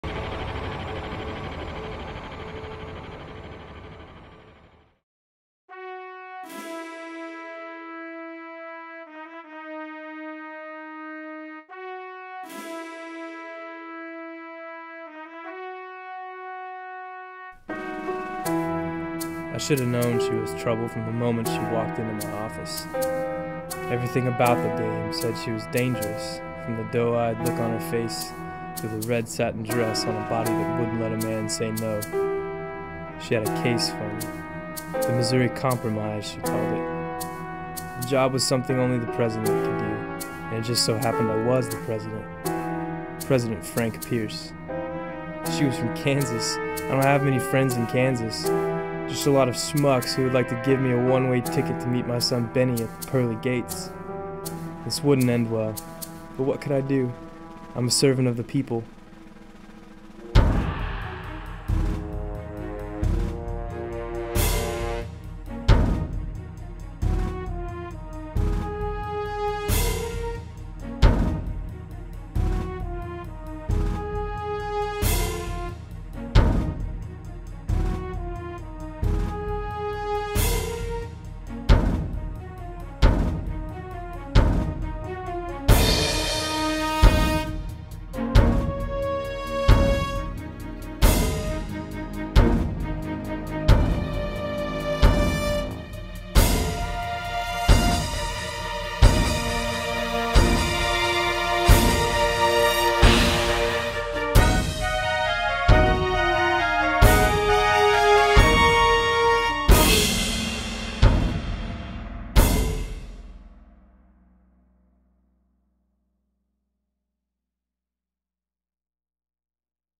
Frank Pierce: Private Eye
Contest Entry for Badass Digest Filmmaking Frenzy: Commander-in-Chief

President Franklin Pierce moonlights as a Private Eye